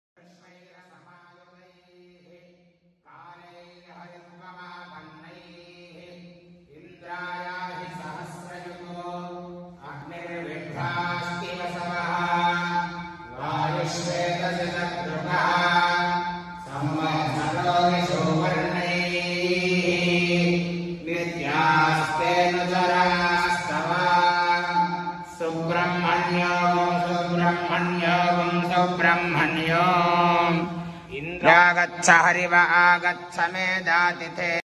గురుపౌర్ణమి సందర్భంగా మిరాలంమండి, శ్రీ మహాంకాళేశ్వర ఆలయం లో జరిగిన దక్షిణామూర్తి ఆరాధన
రక్తాంబరం రక్తవర్ణం రక్తనేత్రాబ్జయుగ్మకమ్
ఛాయా శక్తిసమాయుక్తం ఘృణిసూర్యమహంభజే